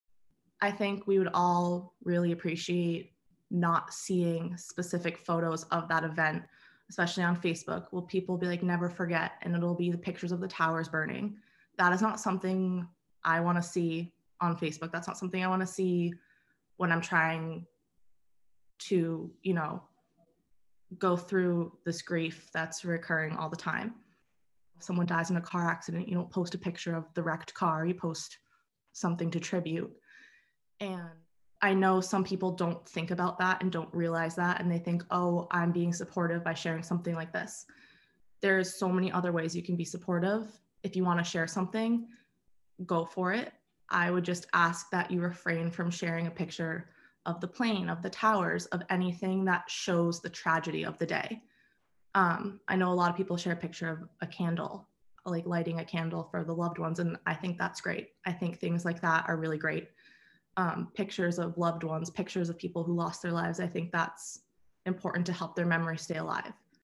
Remembering 9/11: How should general public commemorate 9/11
In this clip, Anna talks about how people who didn’t lose a loved one on 9/11 should think about commemorating the anniversary on social media.

Anna’s mother, Madeline Amy Sweeney, was a flight attendant for American Airlines.  On September 11, 2001, the day our nation was attacked by terrorists, Amy Sweeney was working on American Airlines Flight 11, the first airplane hijacked by the terrorists to devastate New York City by flying into the Twin Towers of the World Trade Center

Background on Amy:

For 14 years, Amy Sweeney was a flight attendant for American Airlines. She lived in Acton, Massachusetts with her husband, Michael Sweeney, and their two small children. On September 11, 2001, Amy Sweeney was working on American Airlines Flight 11, the first airplane that was hijacked and flown into the Twin Towers of the World Trade Center.  Before the plane hit the tower, Amy Sweeney had the courage and bravery to contact the airline’s ground services crew to convey critical information about the five hijackers and their fatal actions on the plane that morning. It is for Amy Sweeney’s heroism, and the heroism and brave spirit of so many of the victims on September 11, 2001, that this award was created.